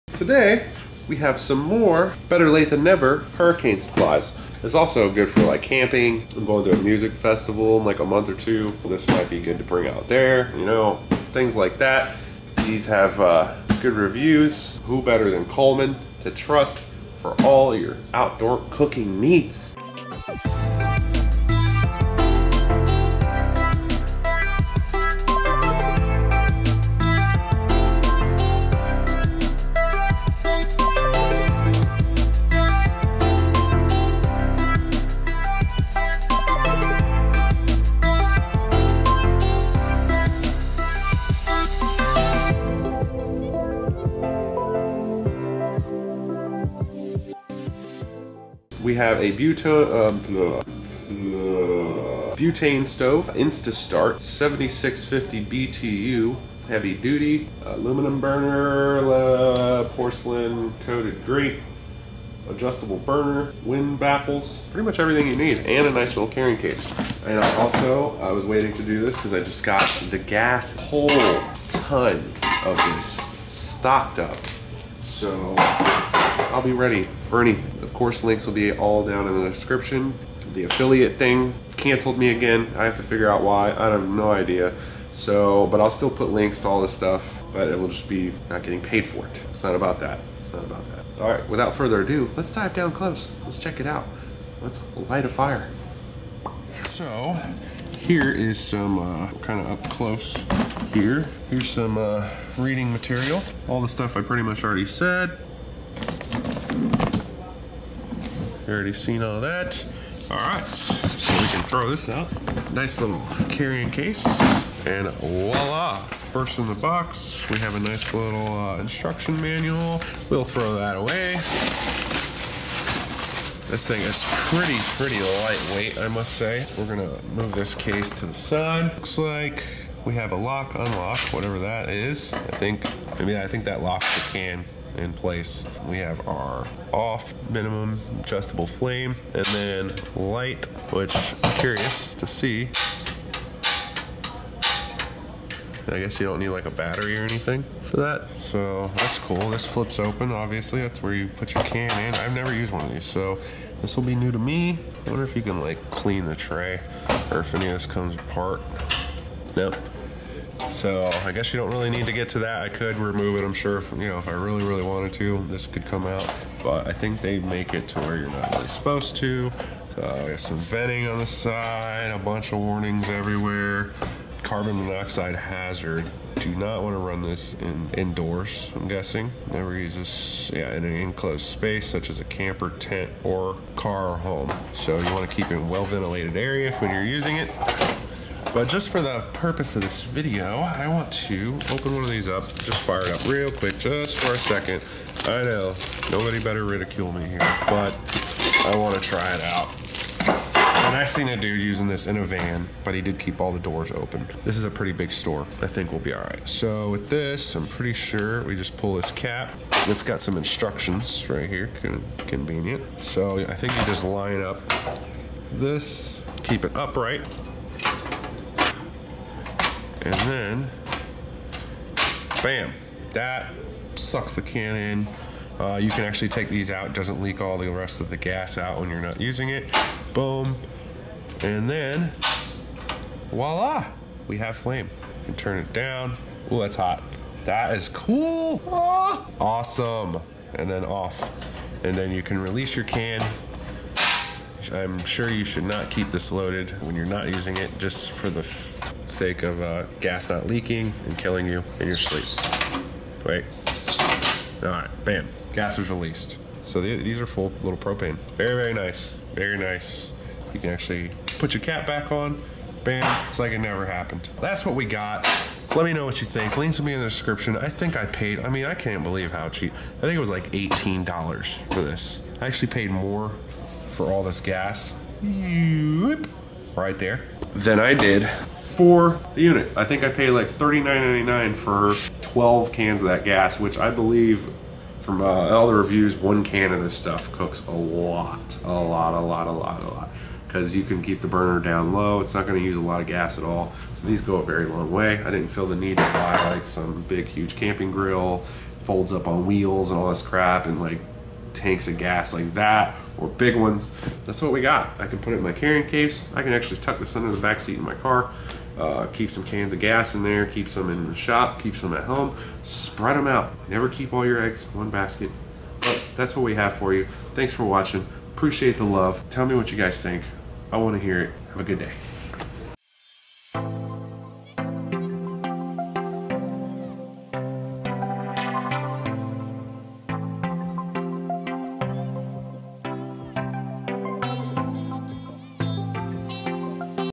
Coleman Butane Stove Unboxing & Fire Up
I just received some more late hurricane supplies from amazon I thought I would share to help everyone stay prepared for future natural disasters. This is a Coleman butane single burner camping stove. Link is below for the gas and stove.